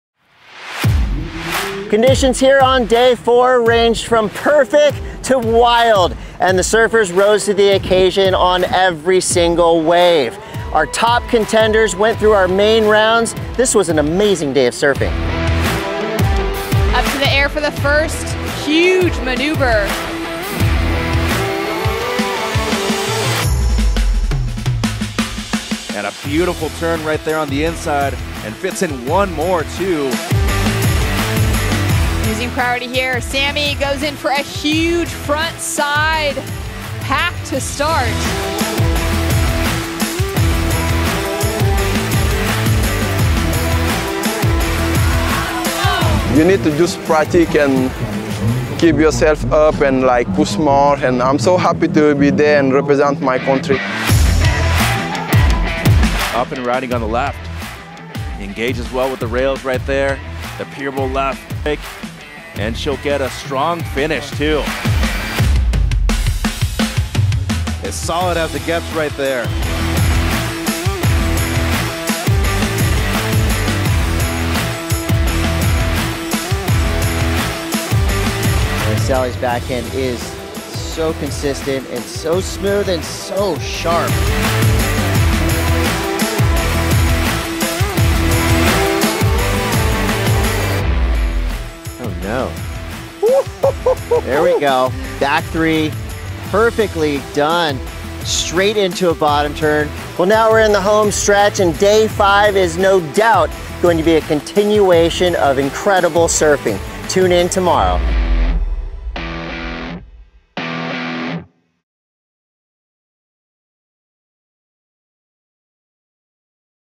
2022 ISA World Surfing Games Competition Day 4 Highlights1
Grandes actuaciones en Huntington Beach en la cuarta jornada del ISA World Surfing Games
Igarashi, los hermanos Pupo y Fitzgibbons con actuaciones dominantes en la carrera por el oro
Cherif Fall hace avanzar a Senegal